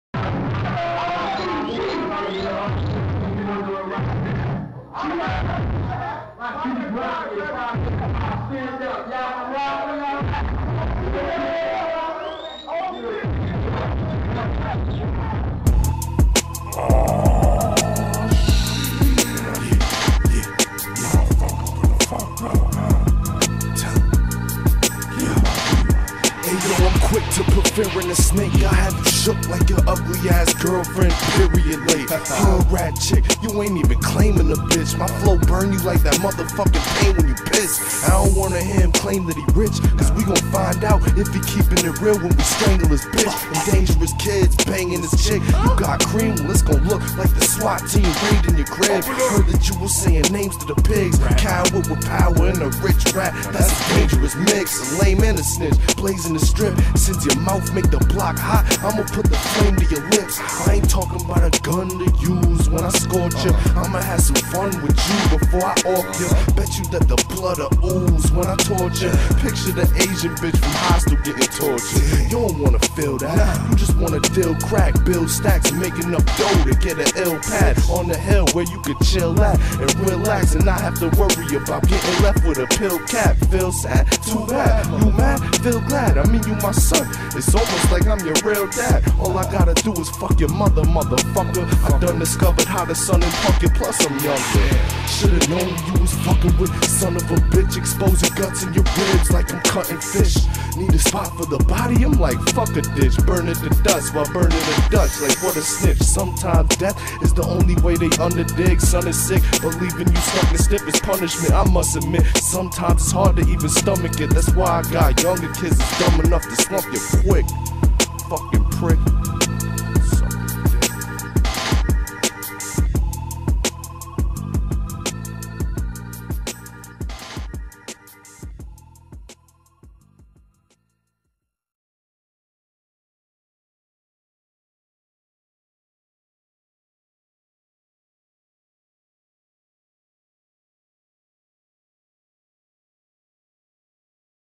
Awww Shiettt
Idenity Solo Jum Off From A Show We Did @ The Penny Arcade IN Rochester Ny